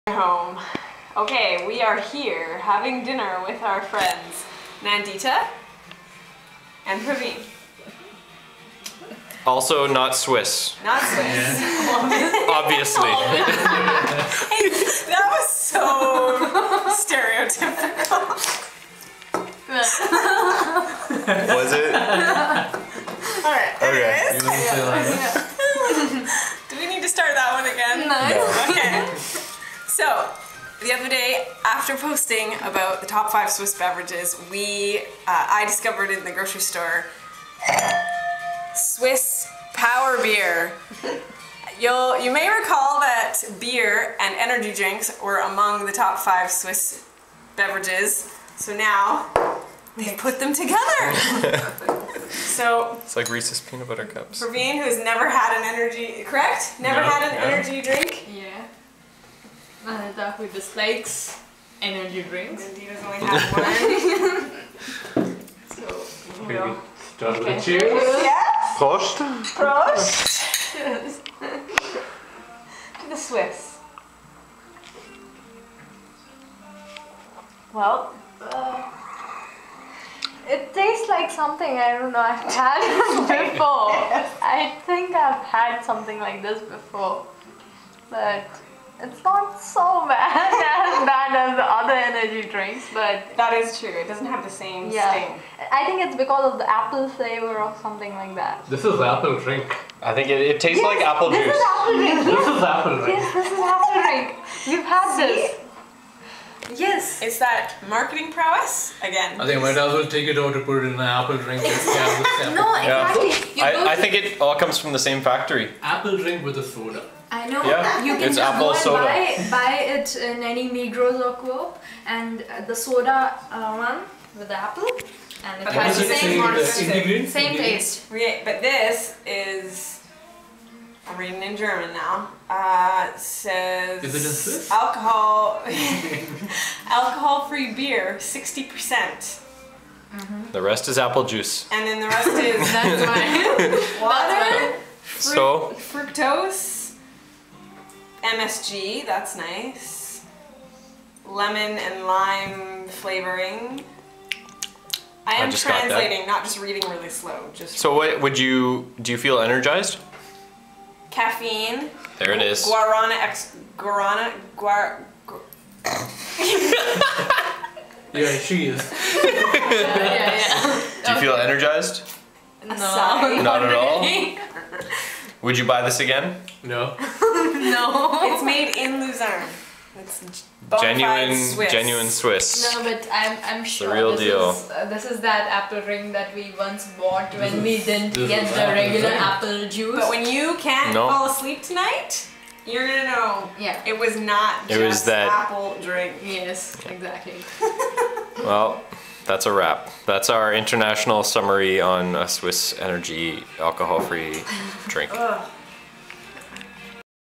Swiss Energy Beer
Beer. Energy Drinks. Two of Switzerland's most popular beverages.  So why not put them together?!